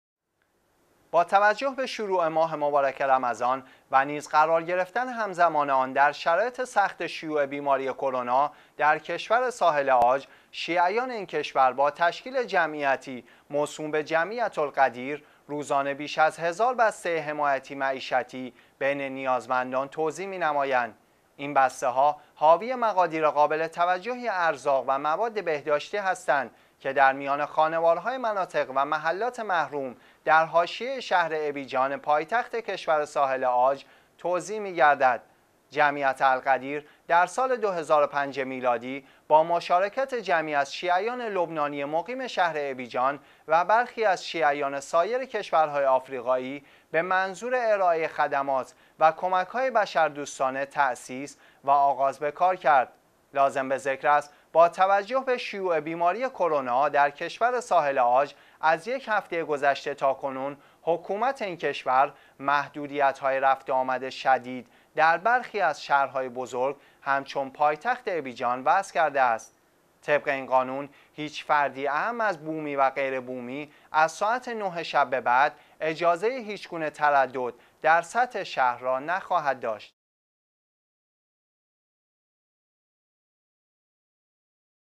توزیع کمک‌های معیشتی توسط شیعیان ساحل عاج در مناطق محروم
با توجه به شروع ماه مبارک رمضان و نیز قرار گرفتن همزمان آن در شرایط سخت شیوع بیماری کرونا در کشور ساحل عاج، شیعیان این کشور با تشکیل جمعیتی موسوم به جمعیة الغدیر، روزانه بیش از هزار بسته حمایتی - معیشتی  بین نیازمندان توزیع می نمایند.
این بسته ها حاوی مقادیر قابل توجهی ارزاق و مواد بهداشتی هستند که در میان خانوارهای مناطق و محلات محروم در حاشیه شهر ابیجان پایتخت کشور ساحل عاج توزیع می گردد.
جمعیت الغدیر در سال ۲۰۰۵ میلادی با مشارکت جمعی از شیعیان لبنانی مقیم شهر ابیجان و برخی از شیعیان سایر کشور های آفریقایی به منظور ارائه خدمات و کمک های بشر دوستانه تاسیس و آغاز به کار کرد.
لازم به ذکر است، با توجه به شیوع بیماری کرونا در کشور ساحل عاج از یک هفته گذشته تا کنون حکومت این کشور محدودیت های رفت و آمد شدید در برخی از شهر های بزرگ همچون پایتخت ابیجان وضع کرده است.
طبق این قانون هیچ فردی اعم از بومی و غیر بومی از ساعت ۹ شب به بعد اجازه هیچ گونه تردد در سطح شهر را نخواهد داشت.